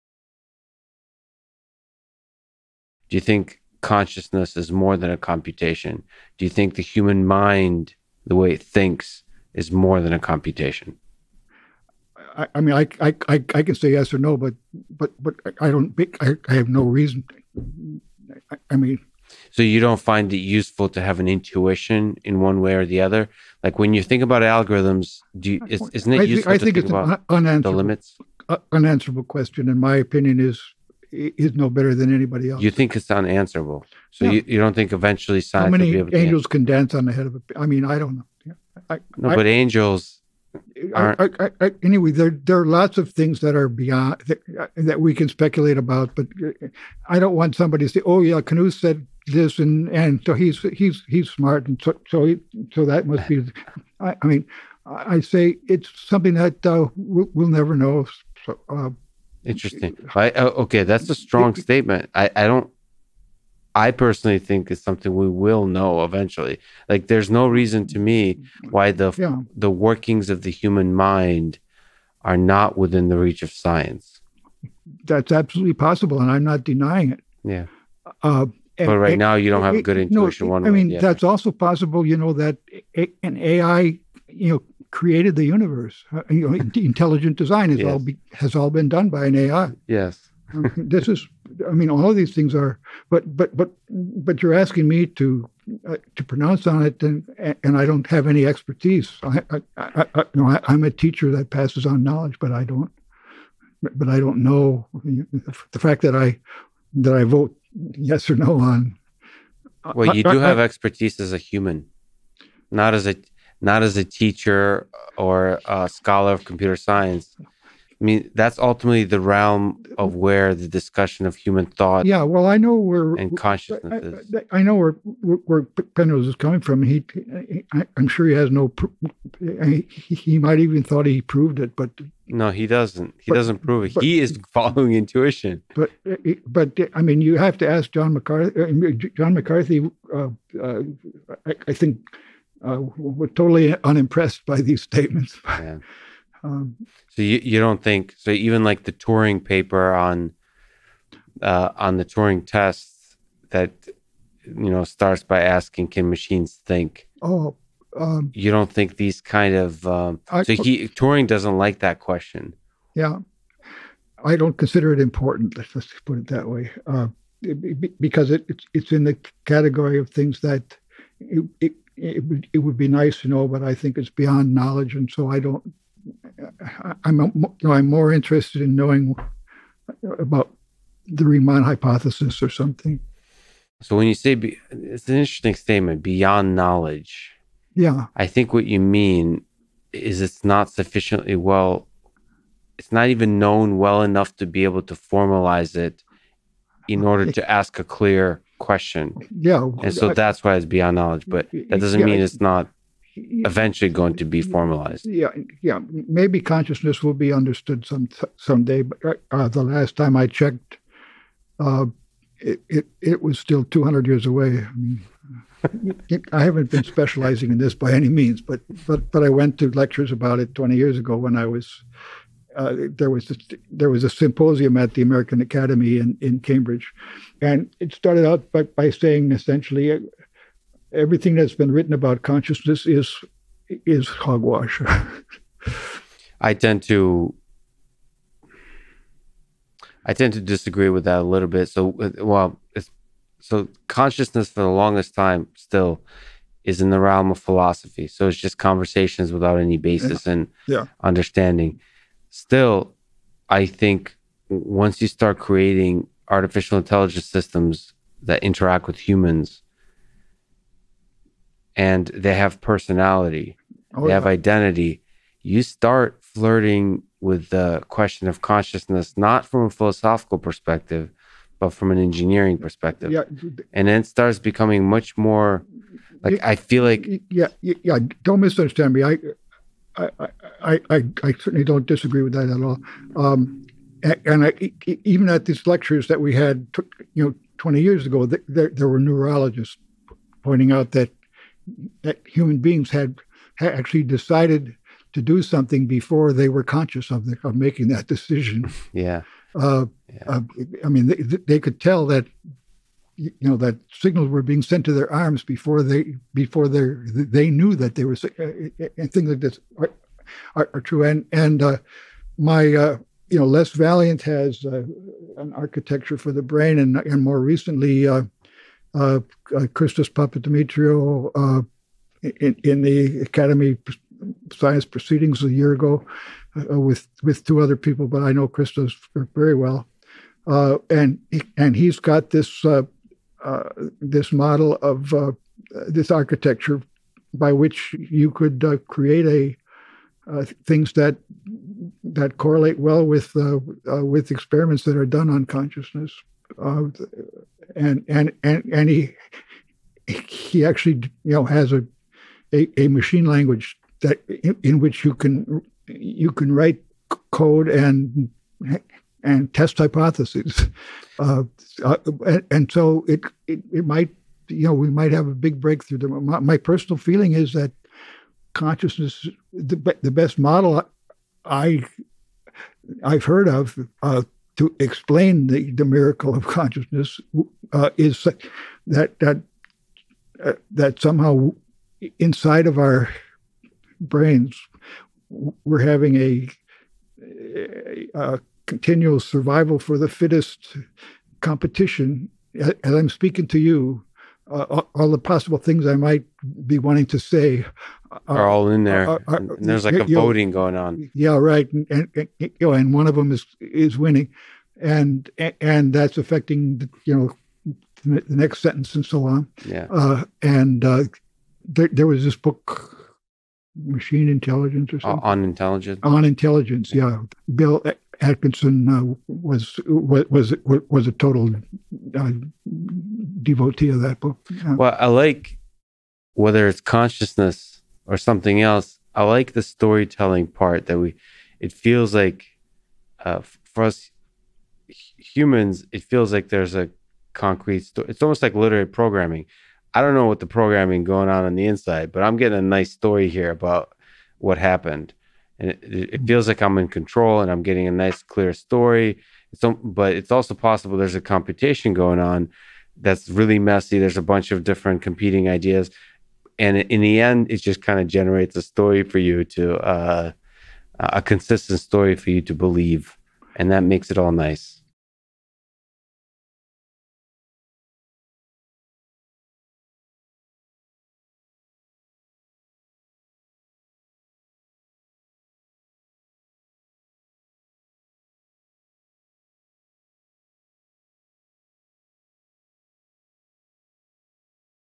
Don Knuth: Is consciousness a computation? | Donald Knuth and Lex Fridman
- Coinbase to get $5 in free Bitcoin

GUEST BIO:
Donald Knuth is a computer scientist, Turing Award winner, father of algorithm analysis, author of The Art of Computer Programming, and creator of TeX.